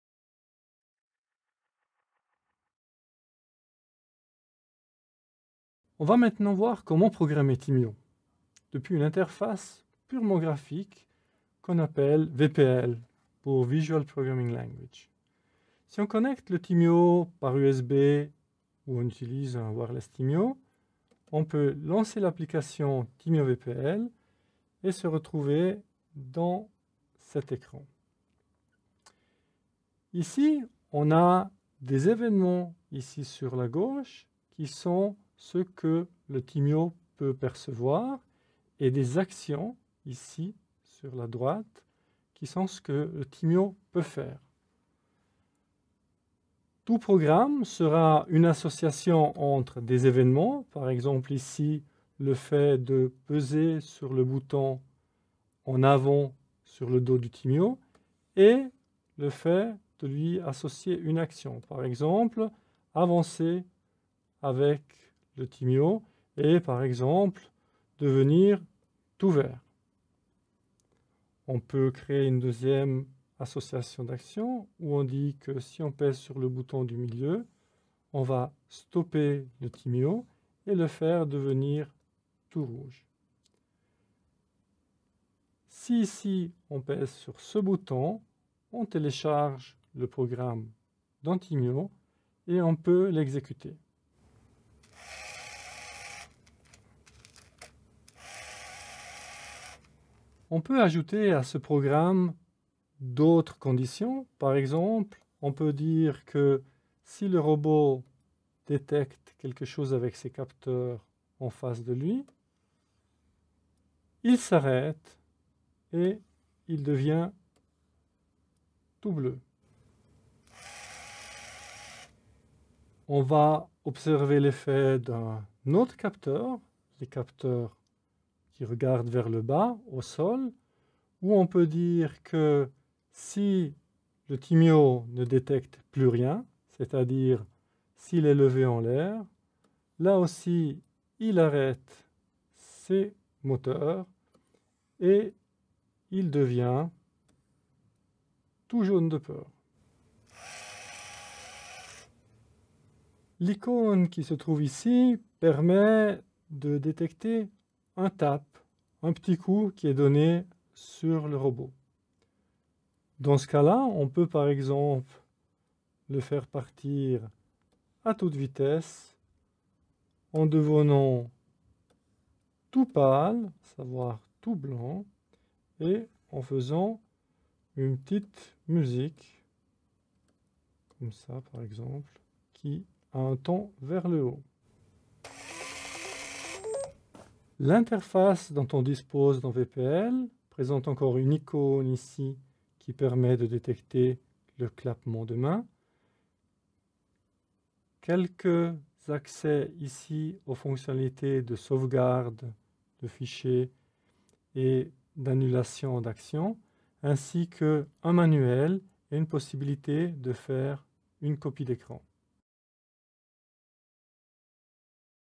Introduction au robot Thymio (2/4) La programmation visuelle VPL
Cette série de vidéos vous fera découvrir le robot Thymio.

Plus précisément, cette deuxième partie vous montrera en détails comment programmer Thymio avec des images grâce à Thymio VPL.

La vidéo suivante vous emmènera dans le mode avancé de Thymio VPL !

Merci à Francesco Mondada pour ces superbes vidéos !